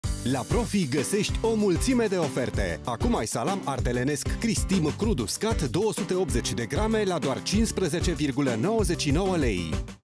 La Profi găsești o mulțime de oferte! Acum ai Salam ardelenesc Cris-Tim 280g la un pret special.
La Profi găsești o mulțime de oferte! Acum ai Salam ardelenesc Cris-Tim crud uscat 280g la un pret special. Oferta supusa unor termene si conditii. Profi. Zilnic preturi mici.